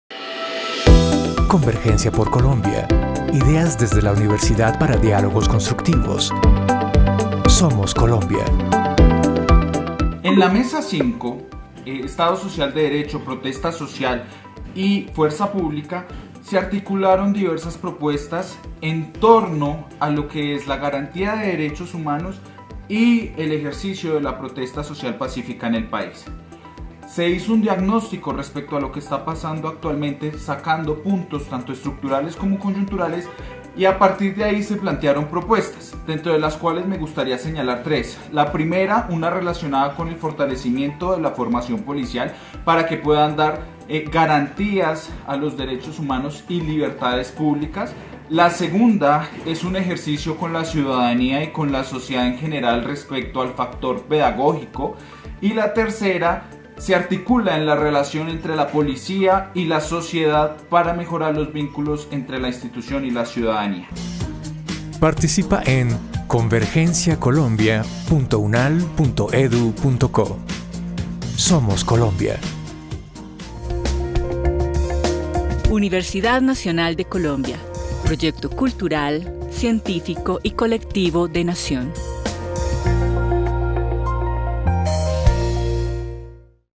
ConvergenciaPorColombia
ConvergenciaPorColombia 🇨🇴 Farid Camilo Rondón, estudiante de la Maestría en Estudios Políticos UNAL nos invita a ser parte de esta construcción colectiva con nuestras propuestas por Colombia ¡Participa! 👉 convergenciacolombia.unal.edu.co